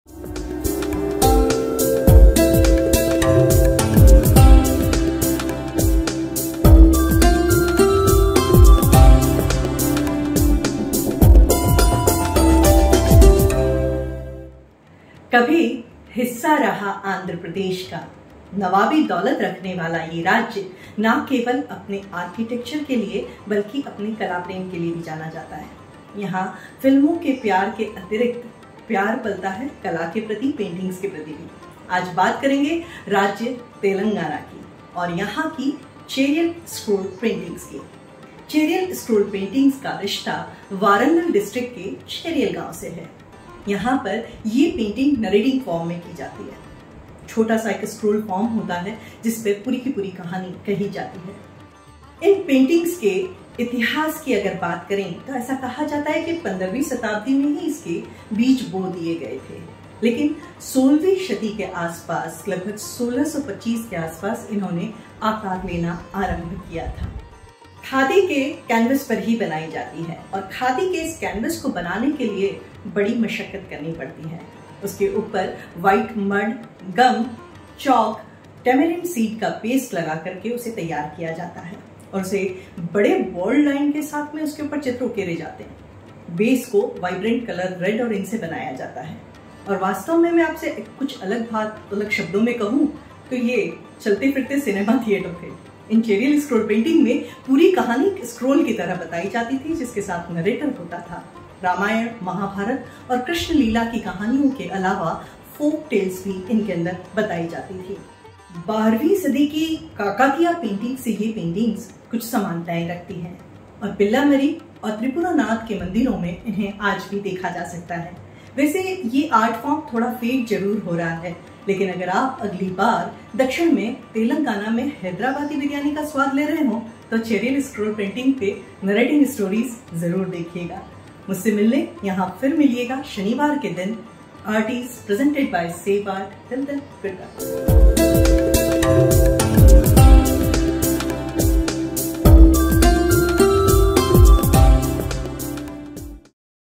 Art Tease Eps- 20 | Cheriyal Scroll Paintings | Telangana | Save Art | SanDeepa | Indian Art-Culture
In today's Art Tease Episode, we're going to unravel the fascinating story of Cheriyal Scroll Paintings, a traditional art form from Telangana, India. We'll explore the intricate designs, vibrant colors, and the rich cultural heritage that these scrolls embody. Join us as we delve into the world of Cheriyal Scroll Paintings and discover the artistry, craftsmanship, and storytelling that makes them so unique.

Cheriyal Scroll Painting is a traditional art form of Telangana, India, that has been passed down through generations. This intricate and colorful painting technique involves painting on a long, thin piece of cloth, which is then rolled up to create a scroll. The scrolls are adorned with vibrant colors, intricate designs, and stories of Telangana's rich heritage. Each scroll is a masterpiece of storytelling, weaving together tales of mythology, folklore, and history. Cheriyal Scroll Painting is a testament to the region's cultural and artistic prowess, and its beauty is unmatched.